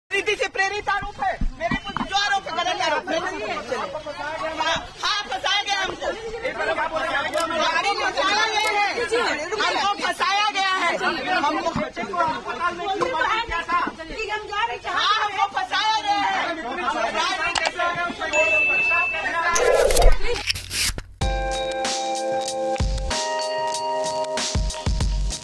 'Politically motivated allegations', says Seema Patra arrested for torturing domestic help
Visuals from Jharkhand show suspended BJP leader, Seema Patra being arrested by the Police after being accused of torturing her domestic help. In the video she can be heard saying "These are false allegations, politically motivated allegations. I have been implicated"